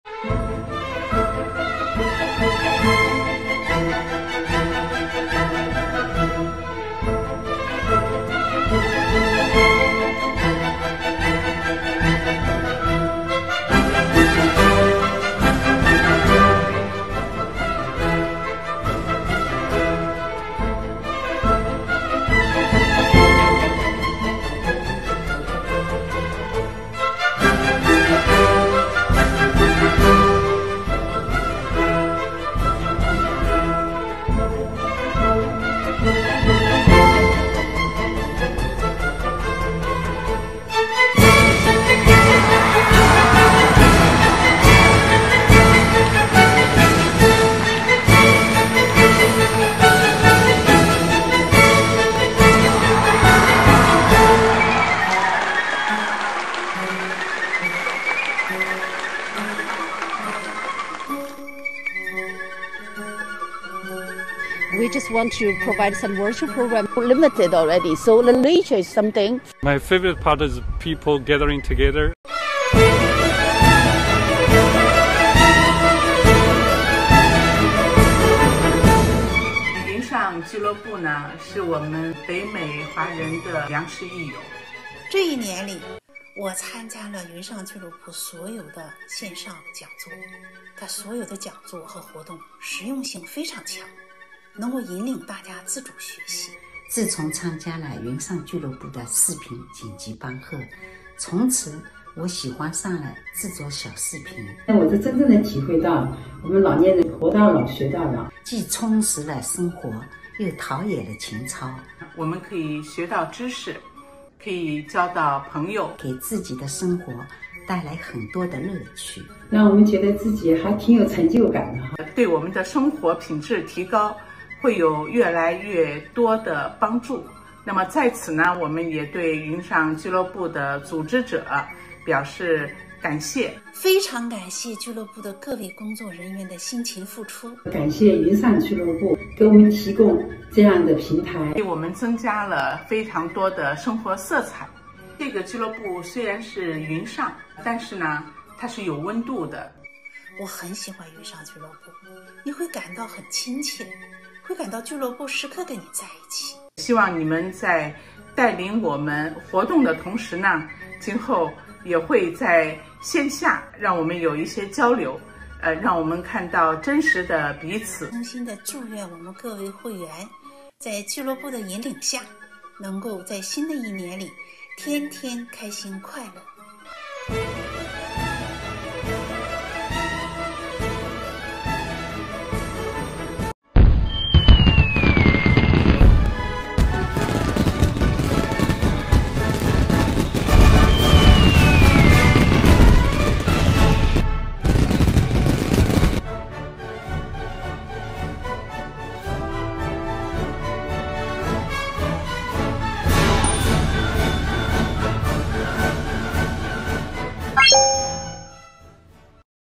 🇨🇦来自云上俱乐部的2023新年祝愿
回眸2022年，云上聚乐部在朋友们的大力支持下，开展了一系列丰富多彩的云上讲座和线下的徒步、一日游活动，还制作了近三百个有关加拿大福利、政策、文化和旅游等信息的视频，CBC NEWS专门对云上聚乐部做了专题报道 2023年，我们将继续努力，我们会继续每月一主题、每周一次的线上讲座活动；提供很多生活信息、解读政府的各种福利政策、及时传递政府发布的应时政策……在前行的路上当然少不了大家的支持和陪伴～欢迎大家邀请亲朋好友加入到我们的大家庭中～